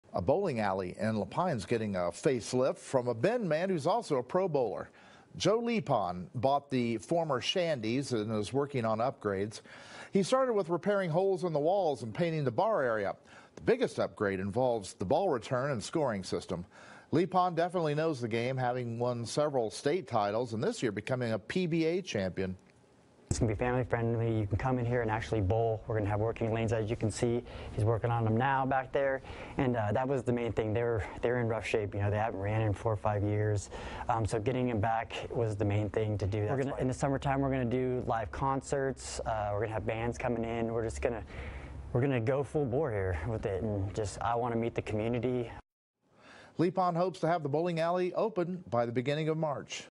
Bend pro bowler reviving La Pine bowling alley
Pro bowler Joe Lipan of Bend bought the former Shandy's bowling alley in La Pine and is bringing it back to life as a fun gathering place for the community.